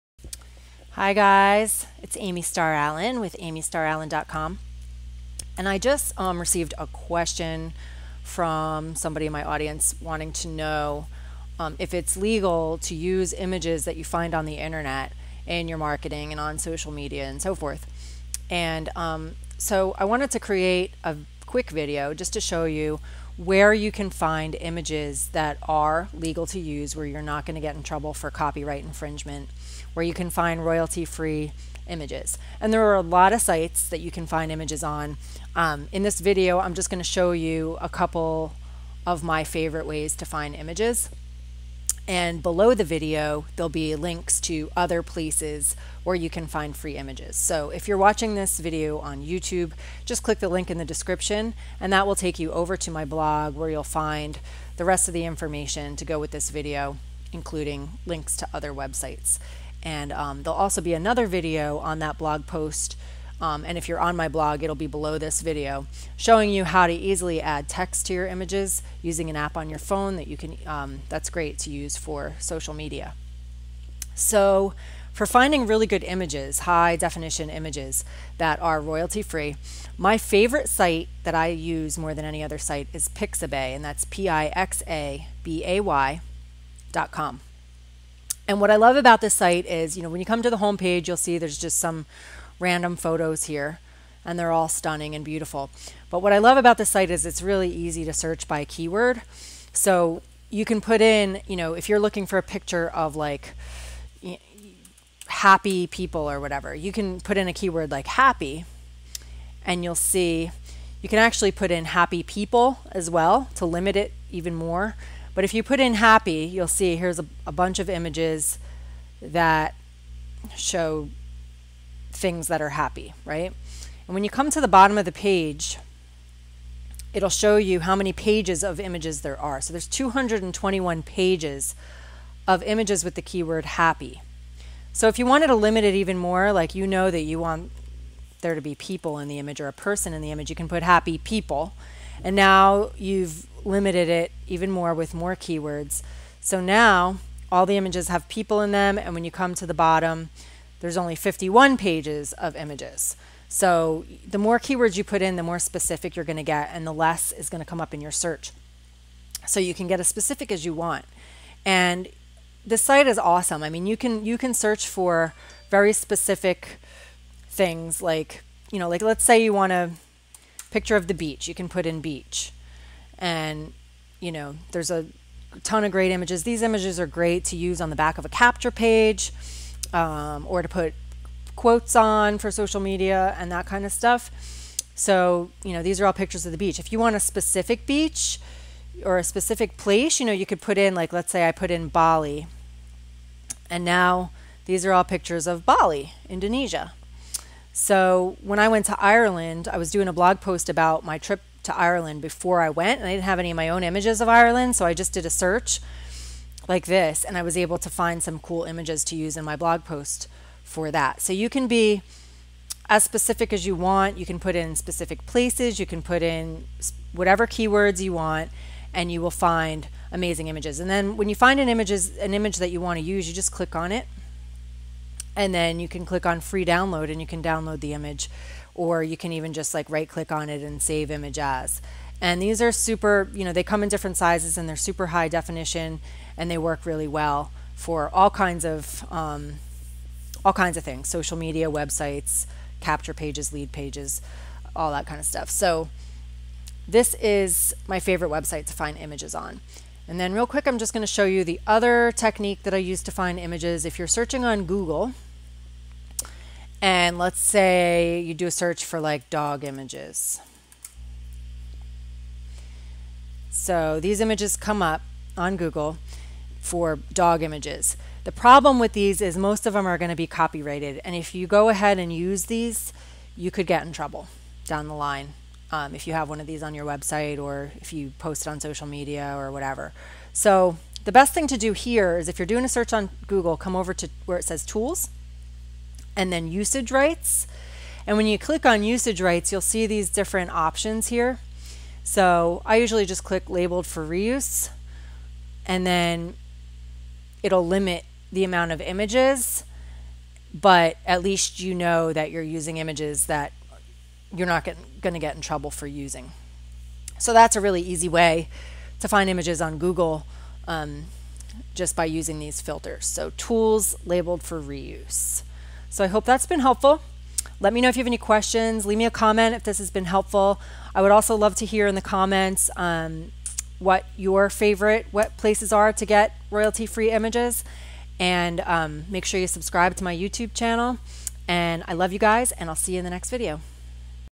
Where to Find Royalty Free Images To Use In Your Marketing
Images are becoming a huge part of branding and marketing these days. They are crucial for social media (Instagram is quickly becoming the go-to platform for marketing and it's all photo based), and it's important to have great images on your website and social media channels as well.

Here's a quick video to show you my favorite places to find images.

Please let me know where you typically go to find quality royalty free images to use in your marketing.

Love you,

Amy

pixabay
free images
marketing images
high definition images
pixabay images
how to find images